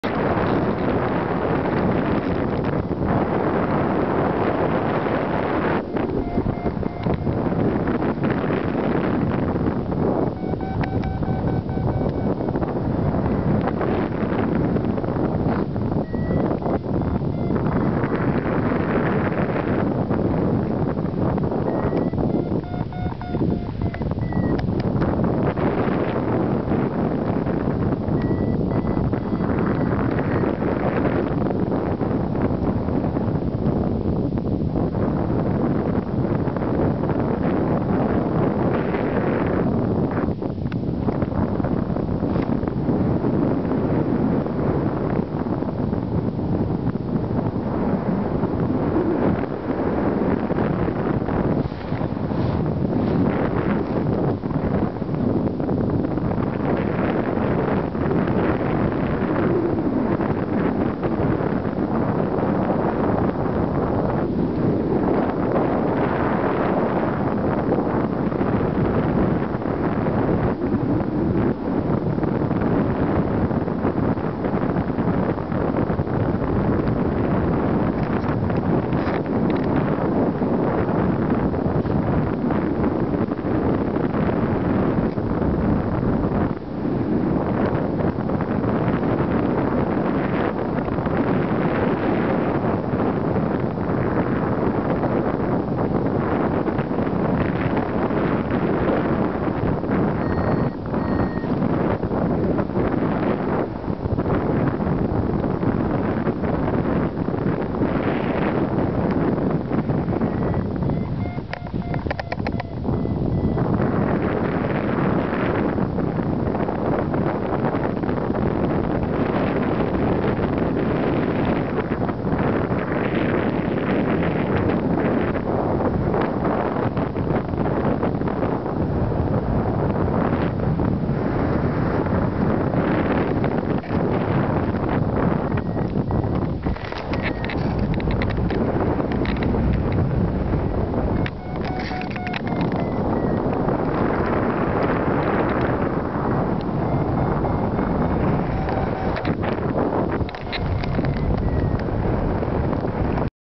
渡り廊下から熊森山へ４.AVI
渡り廊下で目一杯上げて段々熊森山（標高1575m）
を目指す。遠く南アルプスが見えてくる。